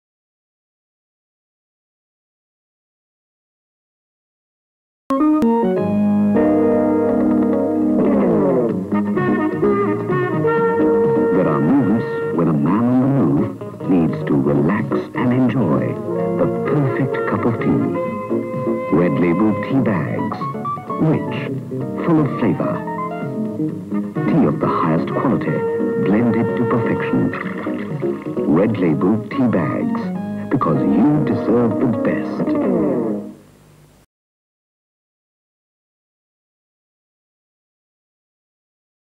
Retro ad for Red Label Tea
Check out this retro ad for Red Label Tea - I'm not sure which country this originated from, but I have taken it from an old UK release Bollywood VHS tape.